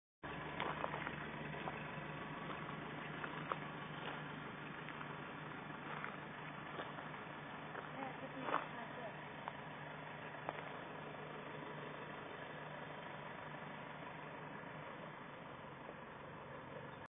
butterflies
spotted these butterflies drivin from tawau to keningau using the upcoming kalabakan sepulut highway